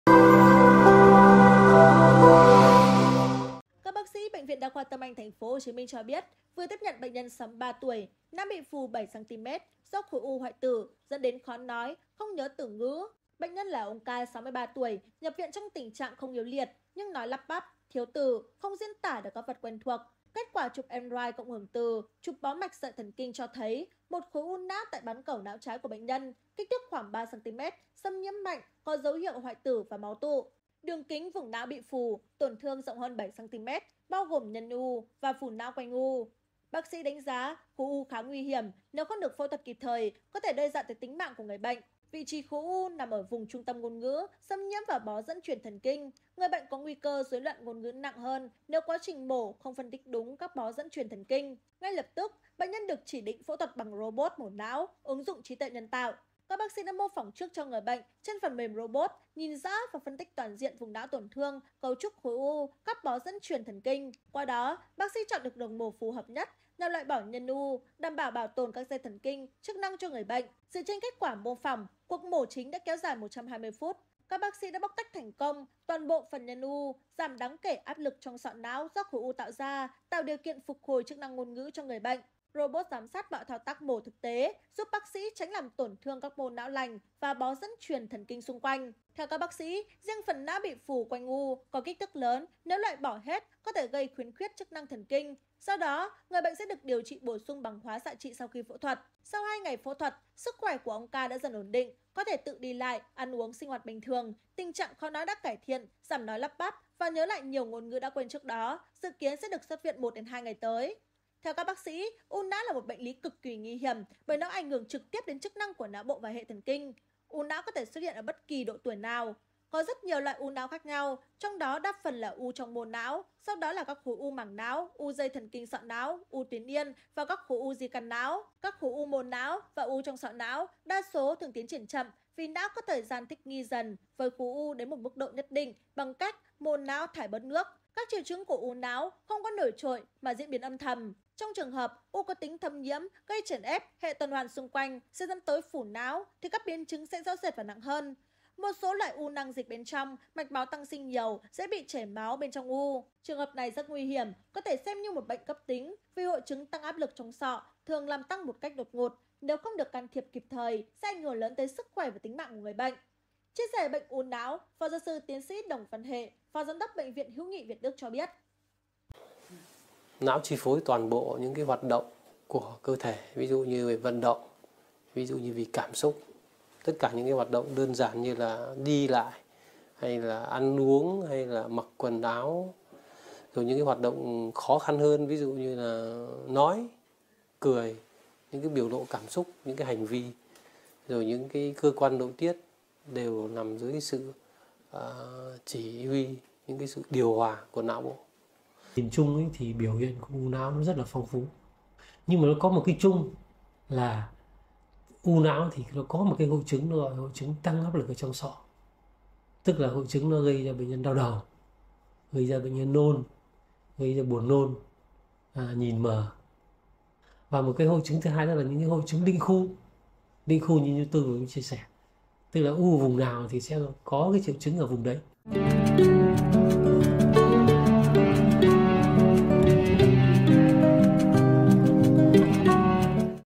Nói Lắp Bắp, Thiếu Từ Phát Hiện Khối U Hoại T.ử Gây Phù Não | SKĐS
SKĐS | Bệnh nhân là ông K 63 tuổi  nhập viện trong tình trạng không yếu liệt nhưng nói lắp bắp, thiếu từ, không diễn tả được các vật dụng quen thuộc. Kết quả chụp sọ não, chụp bó sợi thần kinh (DTI) cho thấy một khối u não tại bán cầu não trái của bệnh nhân, kích thước khoảng 3 cm, xâm nhiễm mạnh, có dấu hiệu hoại tử và máu tụ. Đường kính vùng não bị phù, tổn thương rộng hơn 7 cm, bao gồm nhân u và phù não quanh u.

Mời quý vị và các bạn xem thêm: Nói Chuyện Chậm, Thường Xuyên Phải Dừng Lại Để Suy Nghĩ Bệnh Nhi Lớp 9 Bị Rối Loạn Học Tập | SKĐS